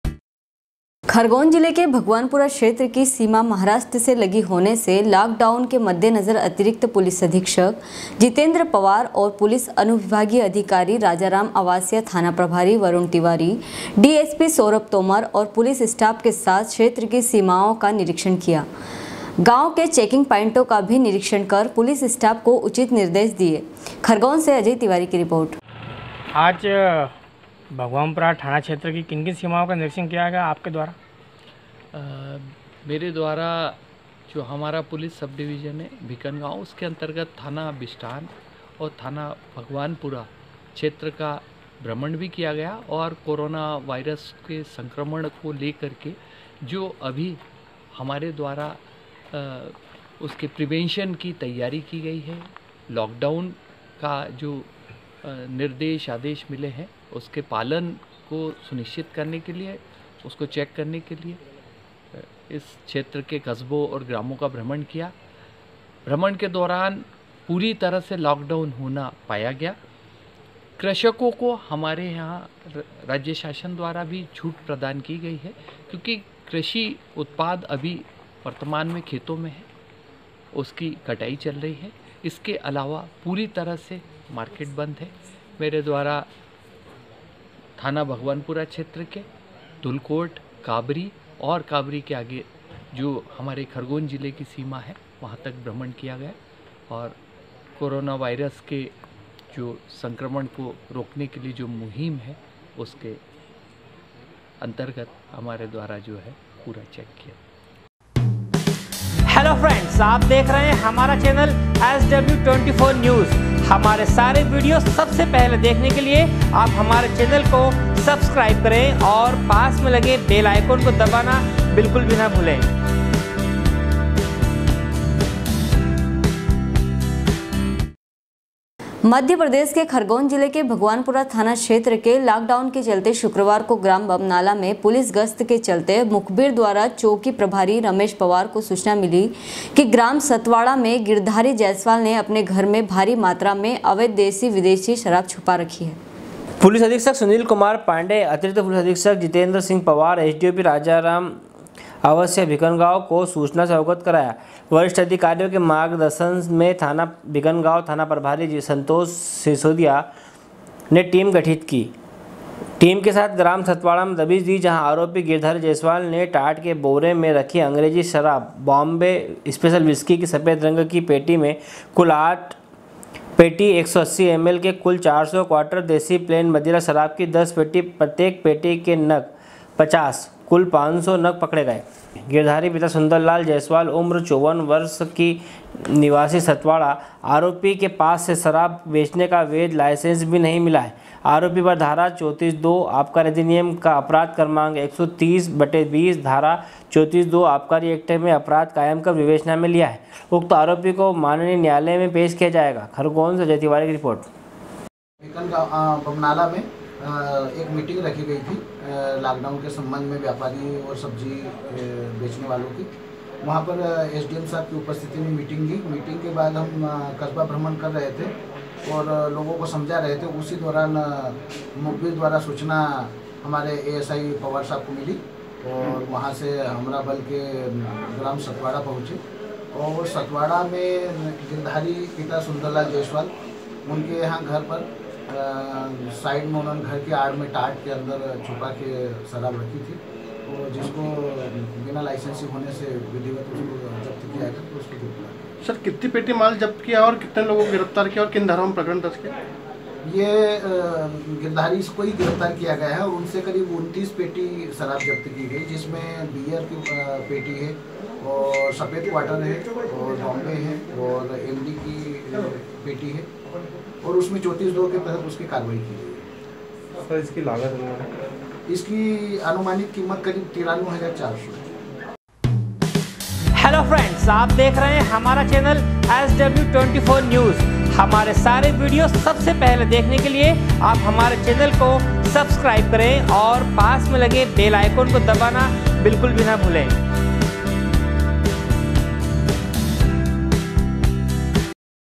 SW 24 News Madhyapradesh#khargone#14.04.2020
sw sw24news sw24newsmadhyapradesh livenews news24मध्य-प्रदेश के खरगोन जिले के भीकनगांव थाना क्षेत्र के लांकडाउन के चलते शुक्रवार को ग्राम बमनाला में पुलिस गस्त के चलते मुखबीर द्वारा चैकी प्रभारी रमेश पवार को सूचना मिली कि ग्राम सतवाड़ा में गिरधारी जायसवाल ने अपने घर में भारी मात्रा में अवैध देशी विदेशी शराब छिपा कर रखी है,
खरगोन से अजय तिवारी की रिर्पोट